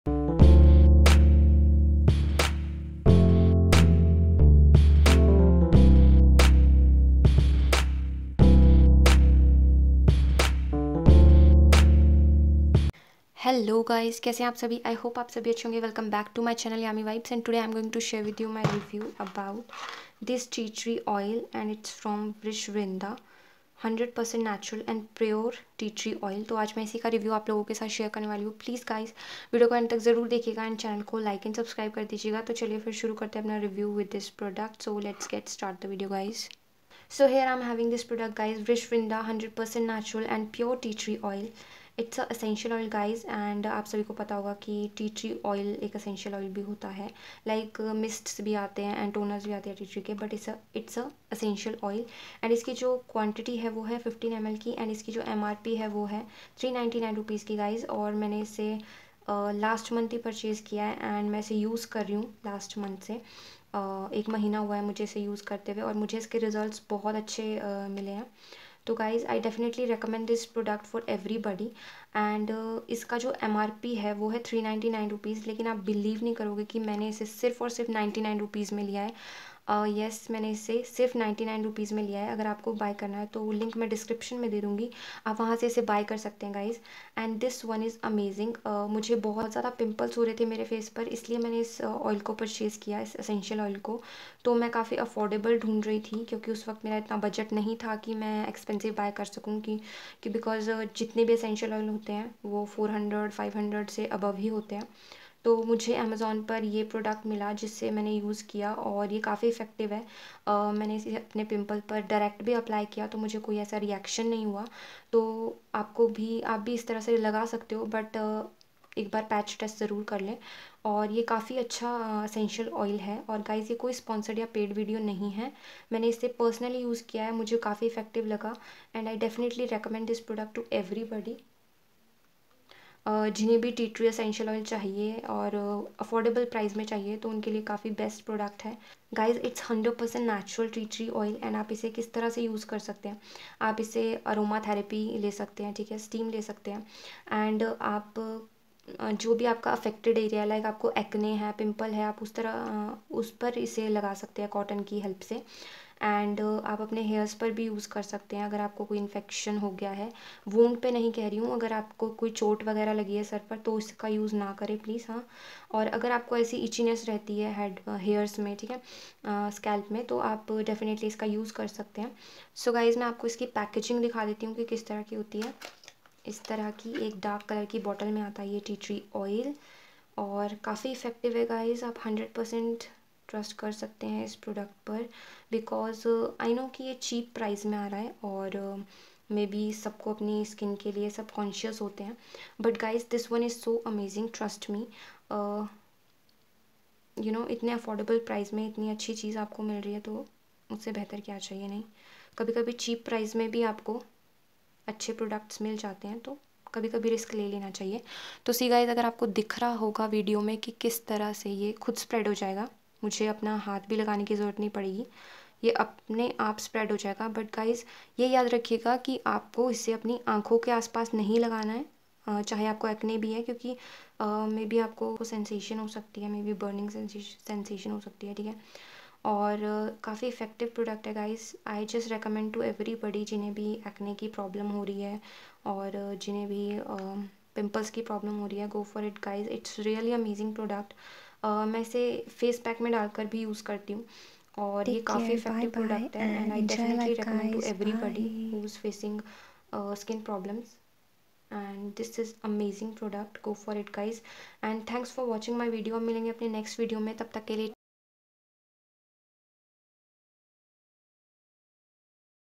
Acne & Pimples ka ek Behtareen Elaaj Reviewing Tea Tree Essential oil
Hello friends

I am bringing  to you Genuine Review  about this product that I am sharing  in this video. Its my own experience while I am using this product.

It's  not a paid or sponsored video it's my personal experience/review  with this product.

So Friends it's my genuine and honest review about this product vrishvrinda Tea tree essential oil. I m using this product from past one month and I am really very satisfy with  their results it works wonderfully. It corrects pimples in just 2 days. I have oily skin and in winters i got lot of  pimples on my face so that's why I buy  it. It's my first purchase at very affordable price.